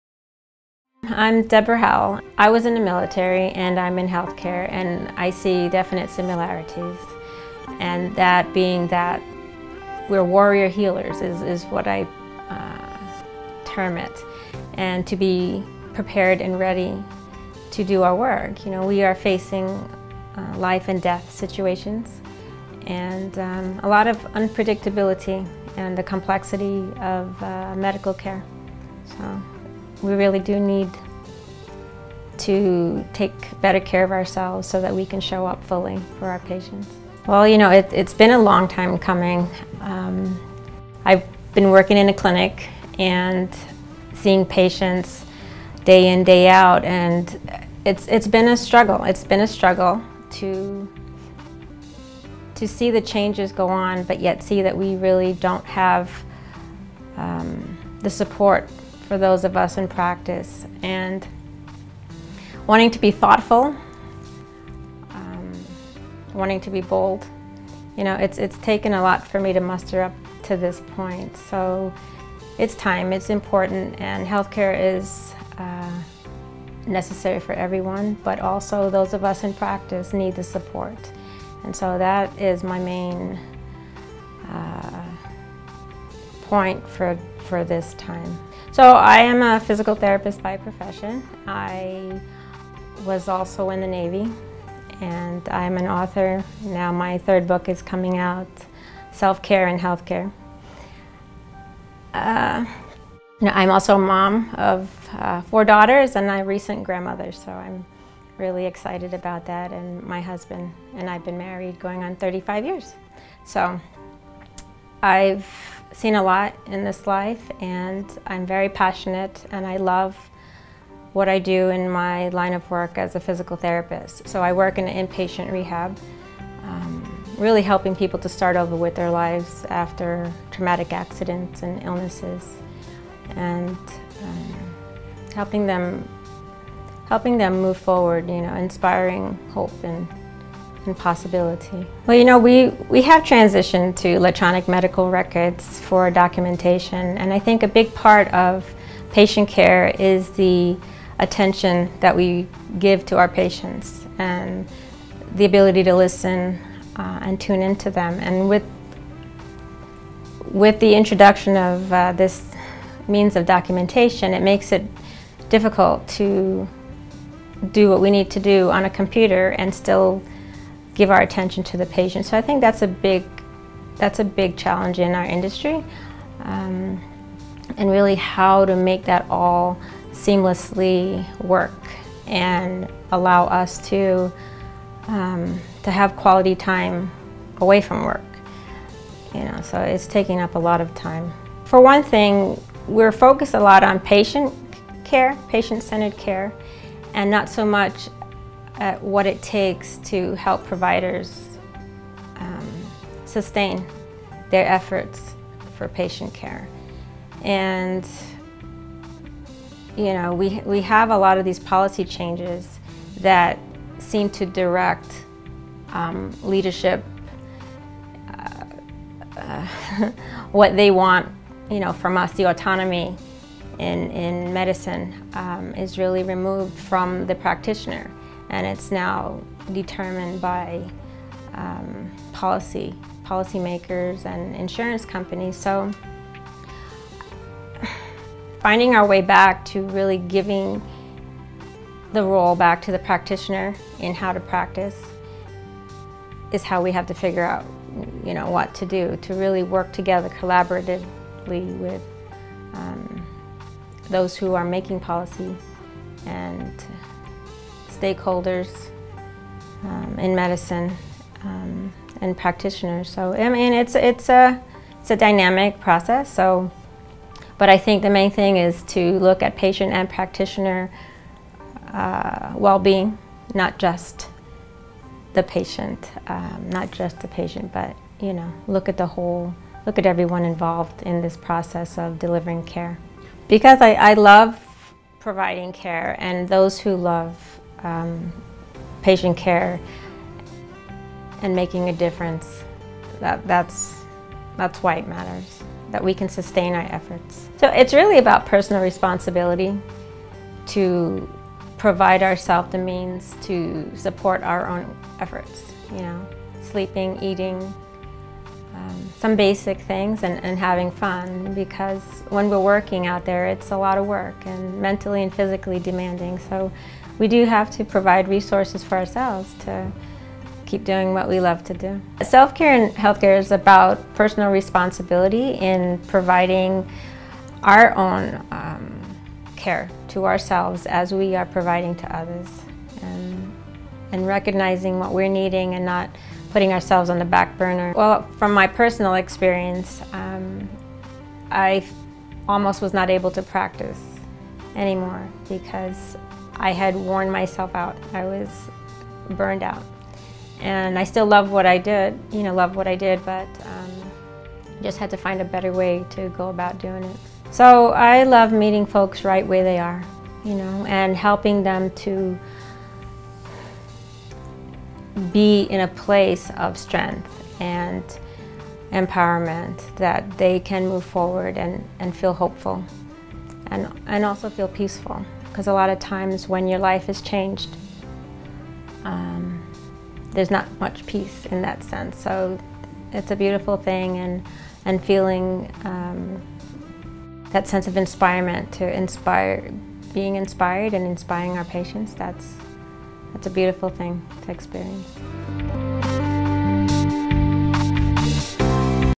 Interview Deborah Howell
Dr. Deborah Howell interview - challenges facing Healthcare providers. Her book Self-Care in Healthcare.